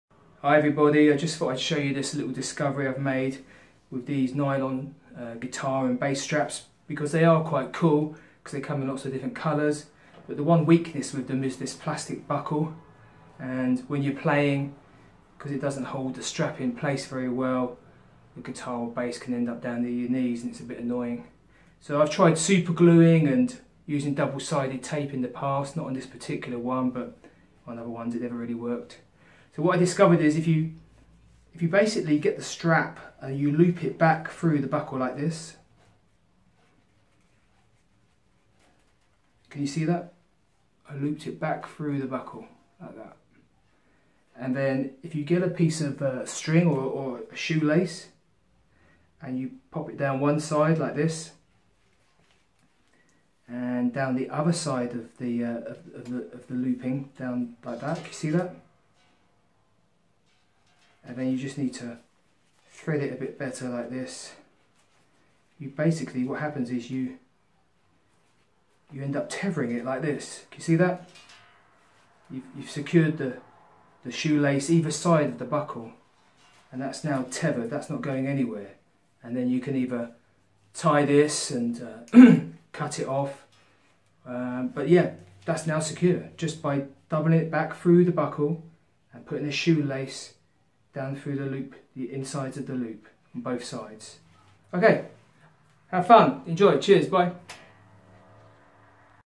Stop nylon guitar strap getting longer
I quite like this silver guitar strap but I don't like the fact it gets longer when I'm playing because the plastic buckle is not very secure, this is very common with these straps. So I discovered that if I looped the strap back through the buckle and tied a shoelace down either side it secured it and solved the problem.